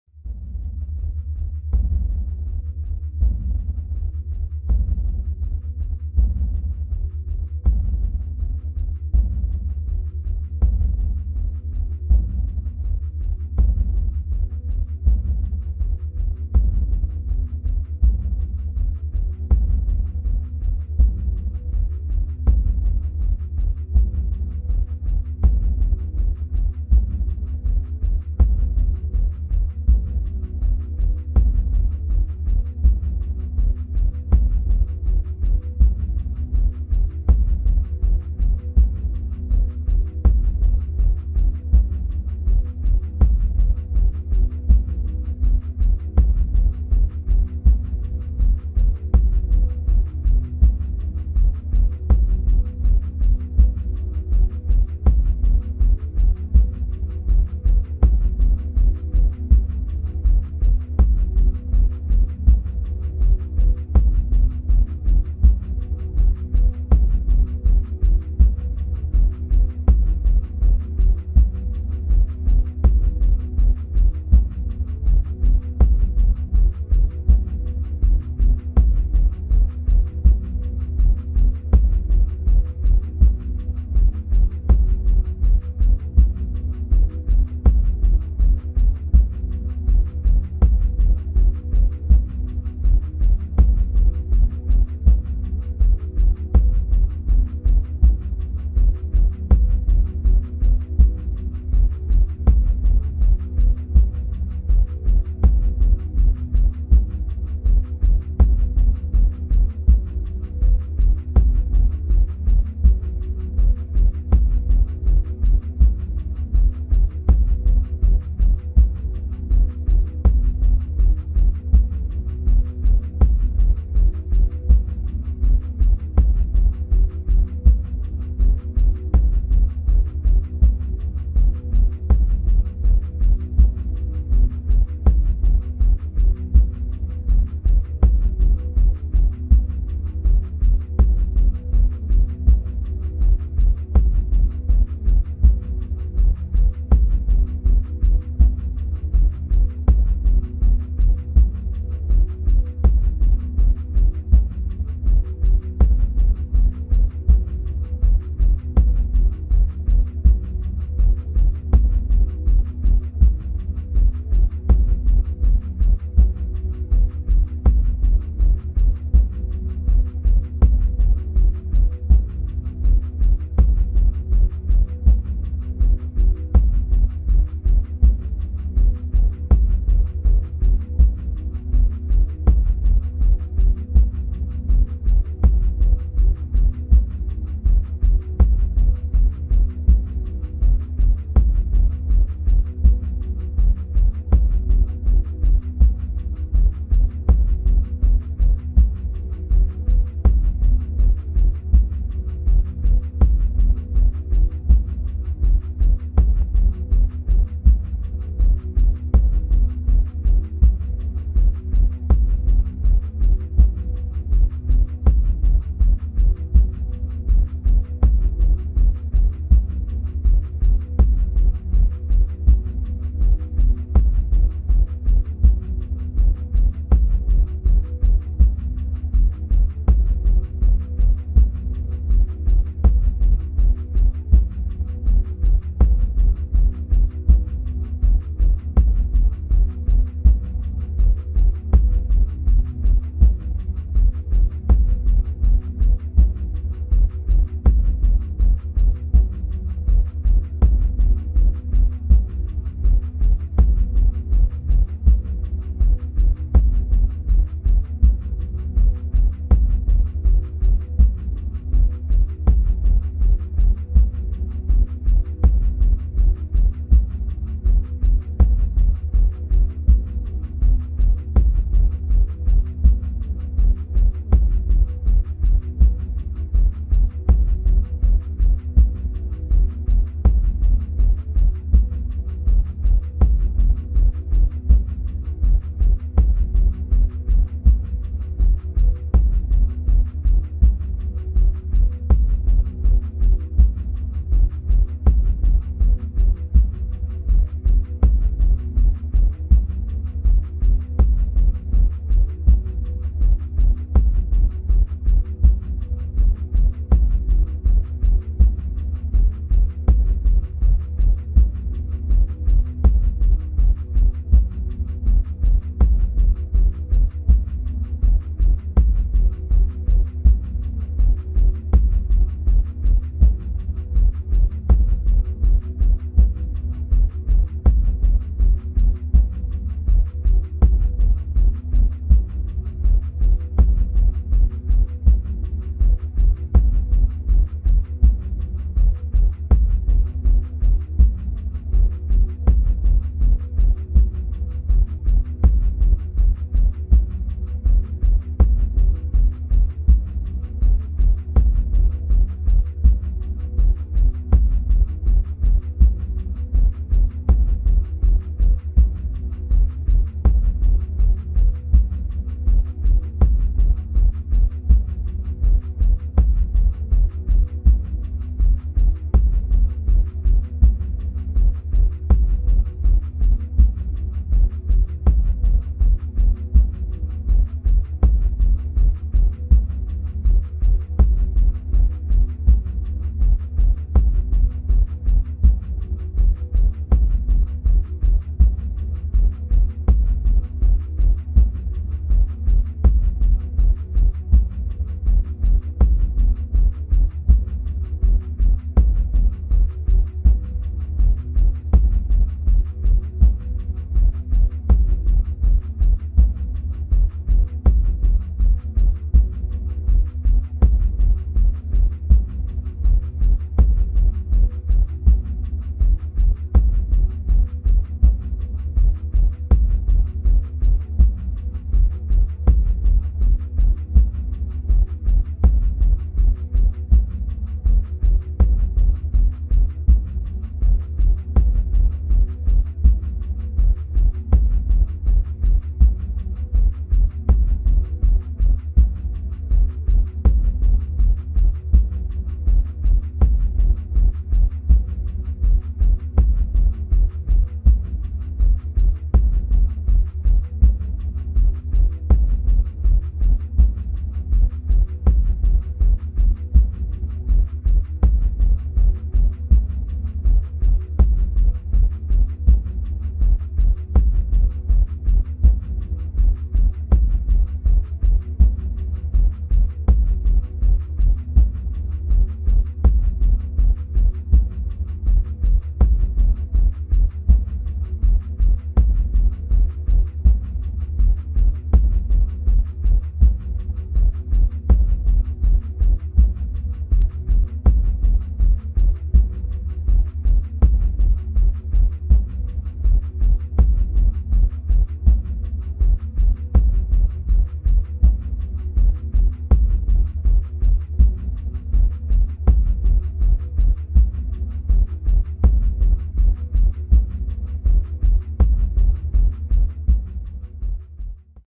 FINALE - SNOWSPEEDER Bandai 1:48 scale build
This finale of the Incom T-47 Snowspeeder from Empire Strikes Back!